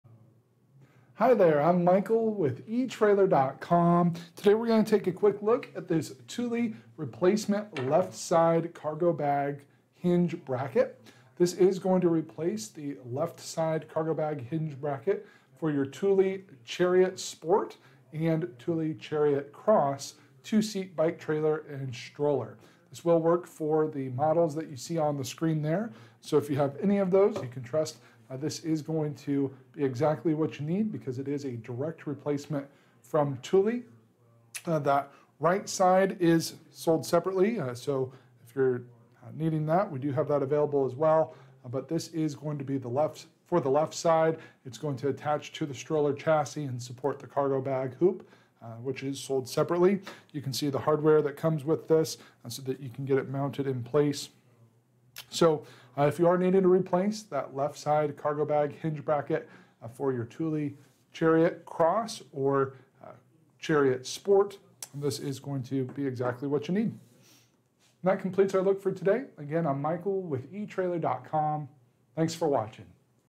Review of Thule Replacement Chariot Trailer and Stroller Left Side Cargo Bag Hinge Bracket - TH98AH
Click for more info and reviews of this Thule Baby Strollers,Bike Trailer For Kids:

Hi there, I'm Michael with etrailer.com.  Today we're gonna take a quick look at this Thule replacement left side, cargo bag, hinge bracket.  This is going to replace the left side, cargo bag, hinge bracket for your Thule Chariot Sport and Thule Chariot Cross two-seat bike trailer and stroller.  This will work for the models that you see on the screen there.  So if you have any of those you can trust this is going to be exactly what you need because it is a direct replacement from Thule.  That right side is sold separately.

So if you're needing that, we you have that available as well.  But this is going to be for the left side.  It's going to attach to the stroller chassis and support the cargo bag hoop, which is sold separately.  You can see the hardware that comes with this so that you can get it mounted in place.  So if you are needing to replace that left side, cargo bag, hinge bracket for your Thule Chariot Cross or Chariot Sport, this is going to be exactly what you need.